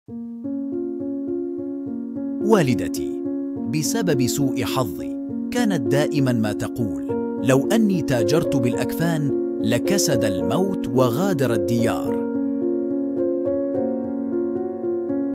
اقتباس للروائي سعود السنعوسي من رواية ناقةُ صالحة
اقتباسات سعود السنعوسي، مقتطفات من رواية (ناقةُ صالحة) للروائي الكويتي سعود السنعوسي.
سعود السنعوسي كاتب وروائي كويتي، عضو رابطة الأدباء في الكويت. فاز عام 2013 بالجائزة العالمية للرواية العربية في دورتها السادسة عن روايته ساق البامبو.

لدعم القناة على بايير payeer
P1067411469

5) PINTEREST :
 pin.it/1lEhBdq‎
6) TUMBLR :